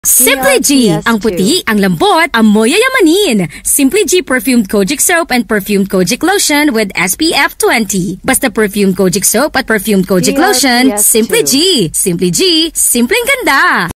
Symply G Prefume Kojic Line Radio Ad Q2 2023 14s (Female Voiceover Version)
Ito po ang Q2 2023 Radio Ad ng Symply G Perfume Kojic Line of Products (Female VO Version)

DRPS Two 3787: Symply G Prefume Kojic Line Radio Ad Q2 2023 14s (Female Voiceover Version)